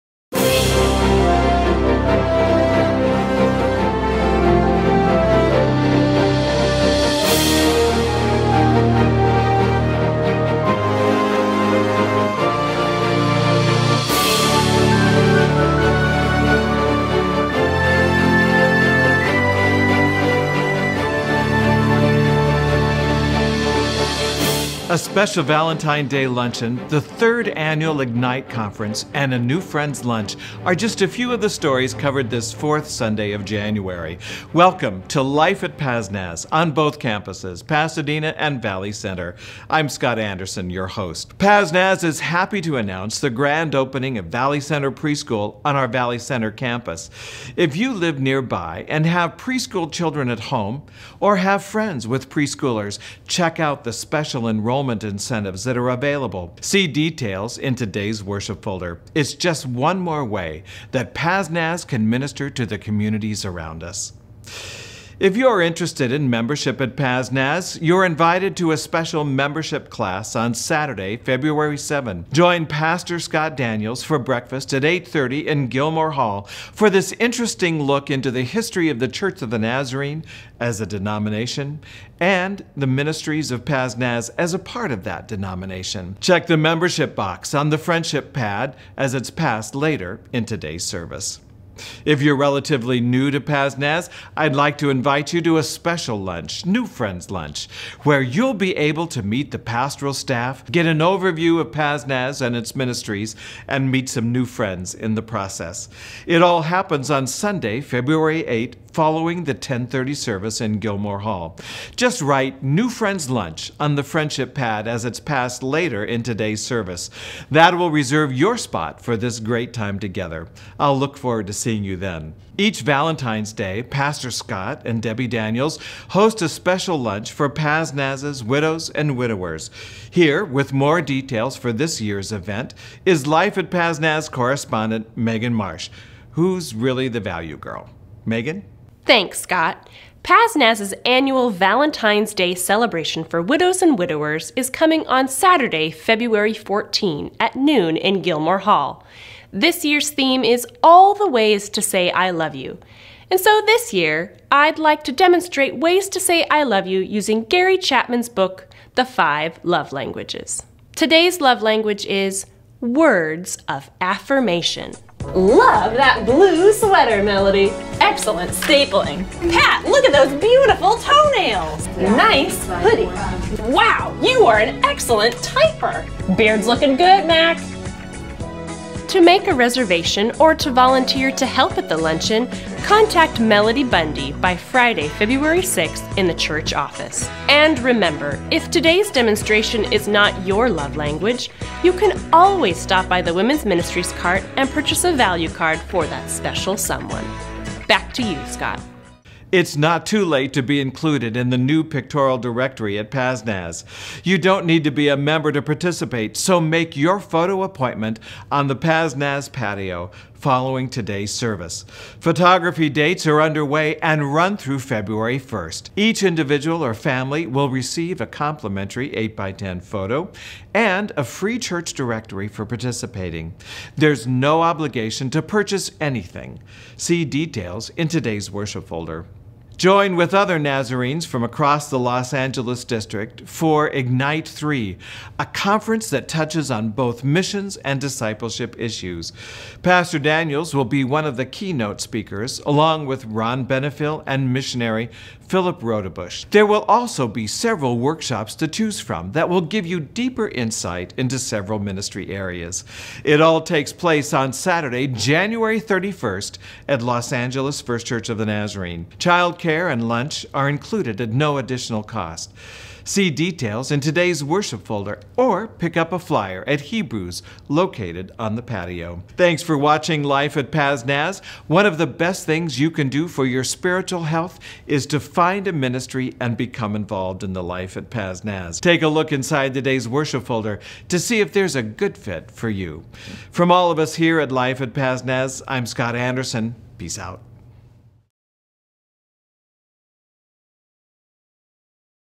Life at PazNaz, January 25, 2015
A Membership Class, New Friends Lunch, and the Valentine's Luncheon are just a few of the stories covered this fourth weekend of January.